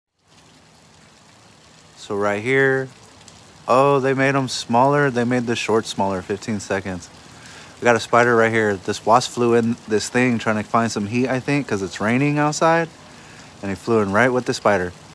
Spider Vs Moth
A Moth got Trapped in a spiders web where it has made a home. The moth was trying to get away from the rain